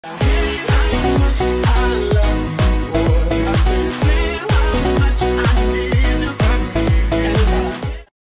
💎 husky siberianhusky dogs puppy dog photography doglover huskypuppy nofilter iphone
Siberian husky puppy. Sunny British weather off leash fun